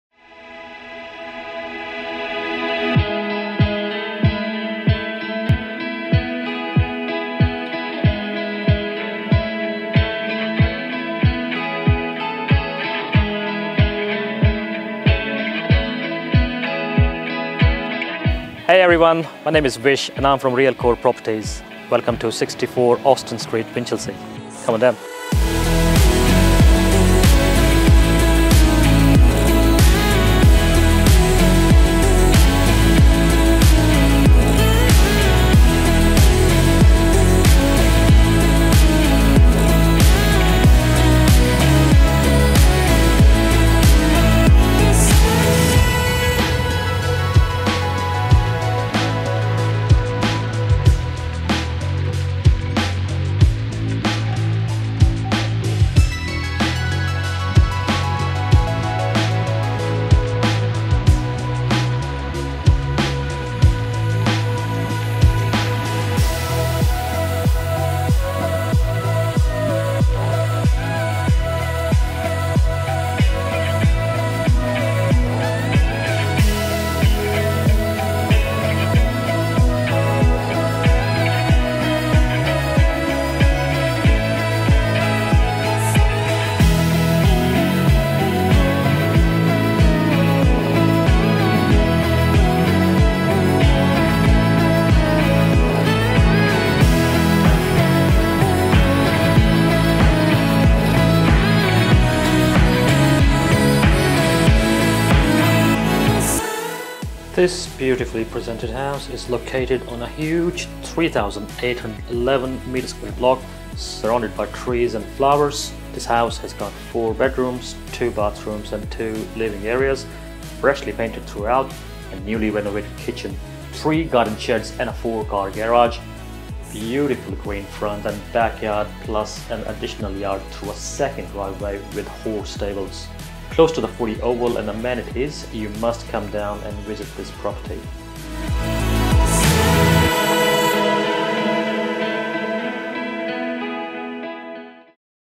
64 Austin Street, Winchelsea, Farm, Development Land, Investment, Real Estate, Acreage, Rural Land
BEST KEPT SECRET IN TOWN.

Located on quiet Austin Street, this well-presented house is set on a massive 3811 m2 block with a big frontage, large backyard through a second driveway that can hold up to 20 cars. You are welcomed by beautiful roses on one side and a shady gumtree on the other side. There is a free-standing gazebo that has clear café-style curtains fenced off from the front yard, perfect for a cup of tea on a winter's day.

Potential to subdivide into units/townhouses (STCA) or live in this neat house as it is, in a very popular pocket of town. Stone's throw-away from the football oval and Princes Highway. Only a few minutes' walk to the town center local cafes, and amenities. Ideal for a horse enthusiast with seven stables in the backyard. Enough room for multiple horse floats.

The house can be easily used as a bed and breakfast for up to 10 people that requires no permit. The large backyard itself can be subdivided (STCA).

Winchelsea is fast becoming a popular destination for people looking for a tree-change. This quaint town with rich history is only 20 minutes away from Waurn Ponds, 30 minutes to Torquay, and 20 minutes to Colac through a dual lane express highway. Just over an hour to Melbourne CBD.

The house includes:

Four generous-sized-bedrooms freshly painted throughout.

Spacious living room upon entry with new log fire heater.

Separate meals/dining area.

Newly renovated kitchen with lot of bench space and storage.

Two bathrooms.

Second living area which has been used as an office in the past.

Alfresco/Outdoor entertainment area.

Four-Car-Garage

Three garden sheds

Seven stables

Backyard divided into two areas, previously used for domestic and commercial use.

Shutters on all windows.

Two split air-conditioning units.

Beautiful garden beds throughout the front and back.

Please see the below link for an up-to-date copy of the Due Diligence checklist: